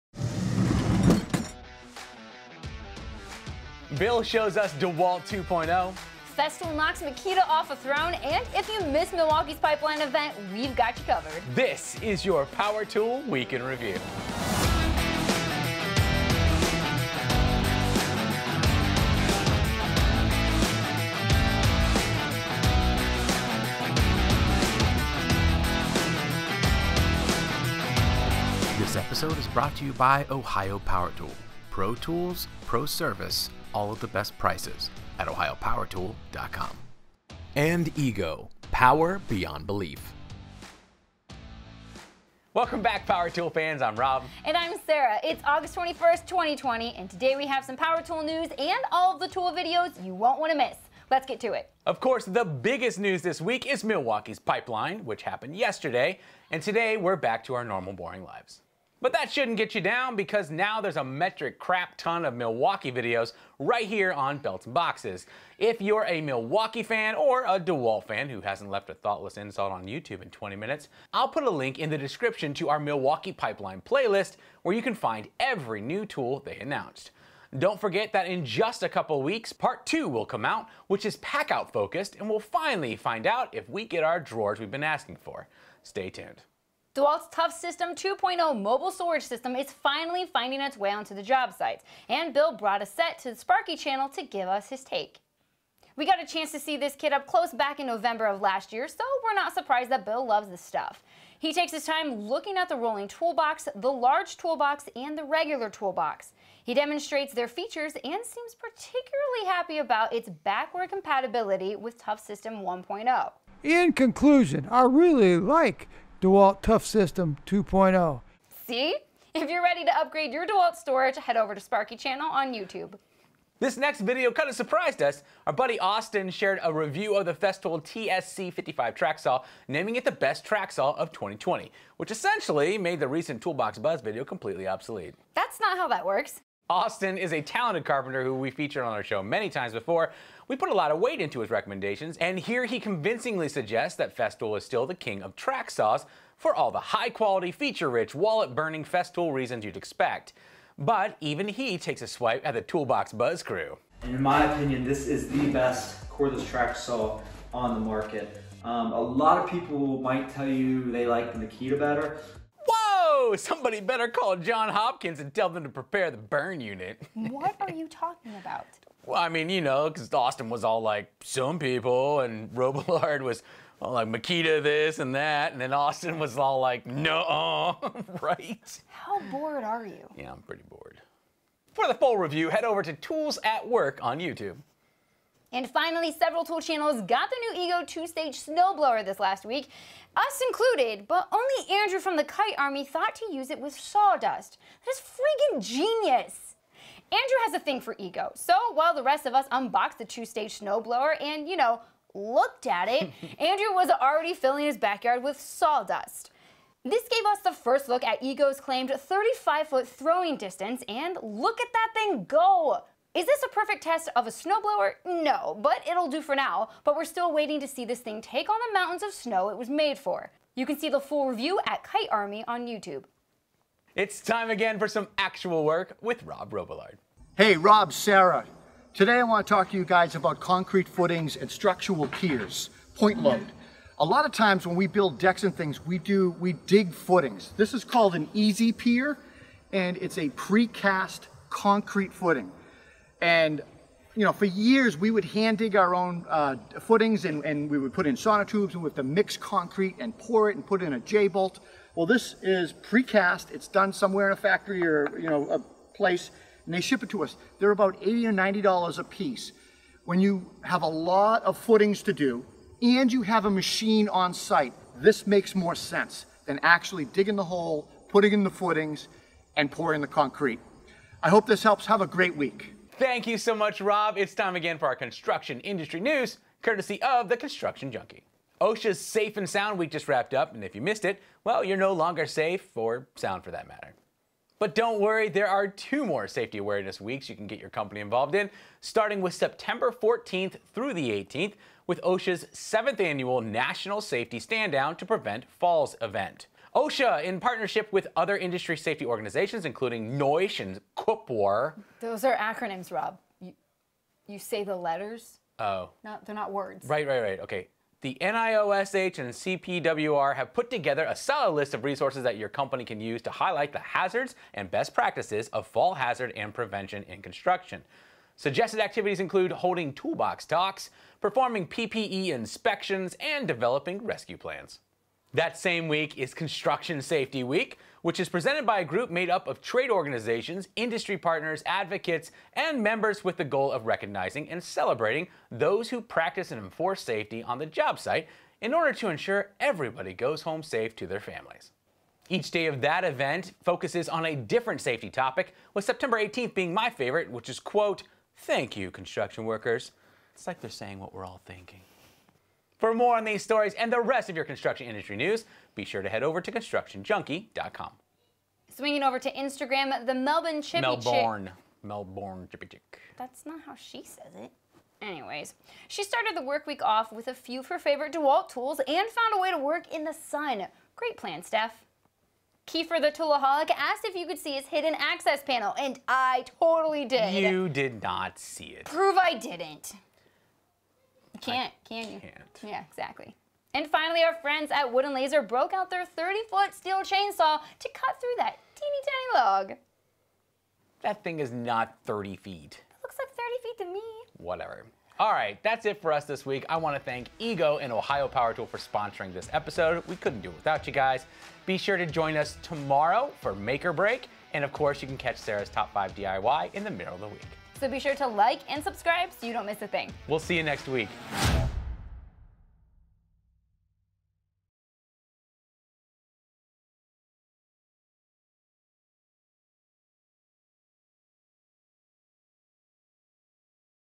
Milwaukee FIGHTS for your attention this week but EGO and Festool fight back. Power Tool NEWS: S3E34
Welcome to Season 3, Episode 34 of your Power Tool Week In Review!

This week Bill from @SparkyChannel shows us DeWALT's 2.0 Tough System, @festoolusa knocks @MakitaToolsUSA off a throne, and if you missed @MilwaukeeTool's PIPELINE event we've got you covered. All of that and more coming up next!

Visit the article on our website where we've embedded every video and Instagram post we mentioned in our show, all on one page!